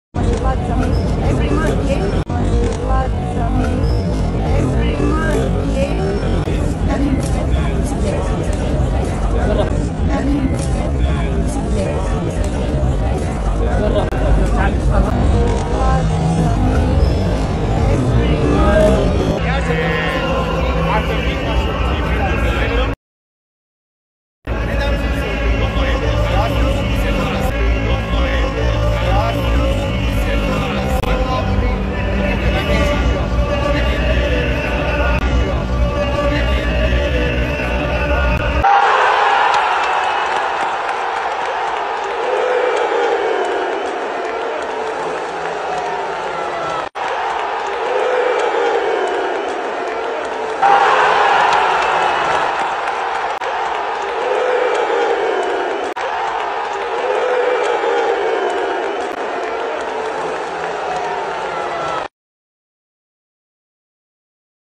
Cristiano Ronaldo jr Arrive To Watch Real Madrid vs Barcelona at the El Classico Match
Cristiano Ronaldo junior arrive to watch Real Madrid vs Barcelona El Classico Match